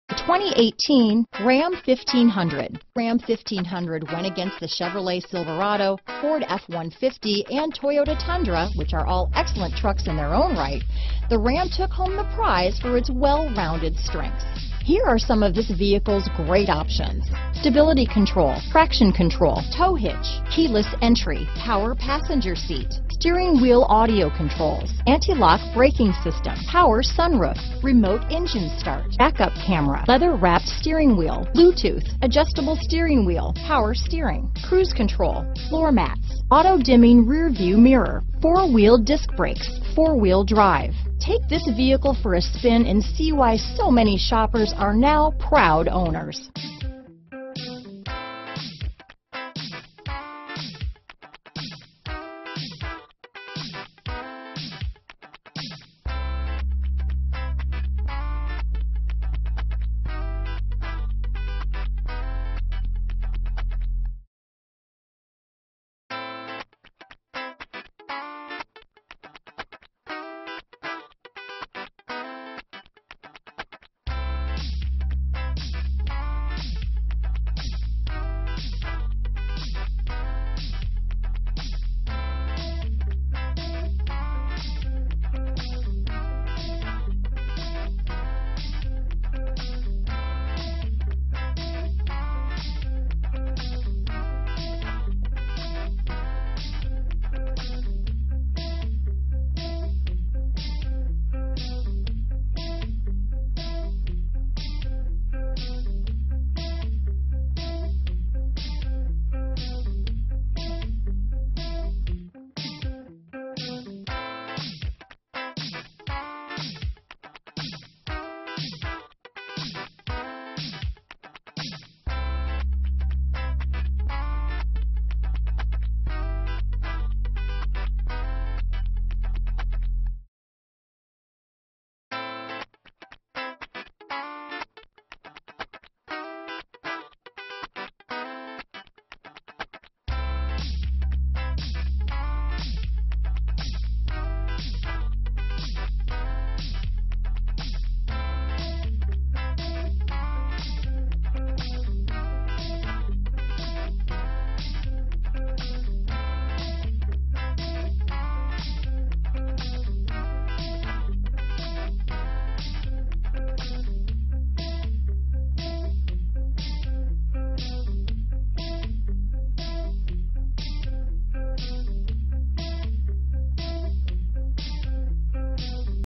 2018 Ram 1500 Boulder, Longmont, Broomfield, Louisville, Denver, CO 15606
Brilliant Black Crystal Pearlcoat New 2018 Ram 1500 available in Boulder, Colorado at Valley Dodge. Servicing the Longmont, Broomfield, Louisville, Denver, CO area.

Boulder Dodge
2100 30th St